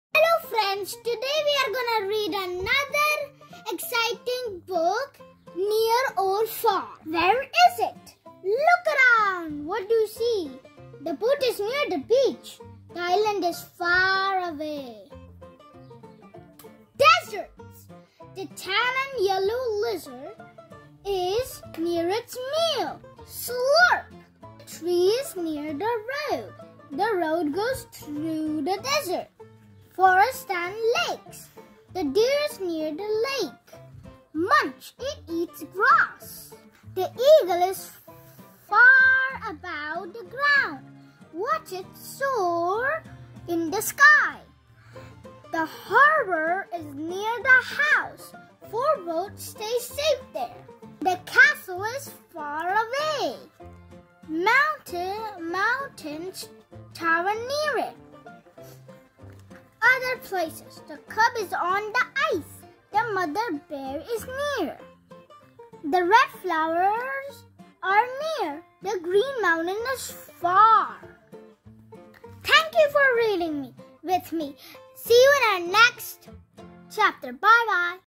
Near or Far
Location words
Is the deer near the lake?
Is the mountain far away?Look inside to find out!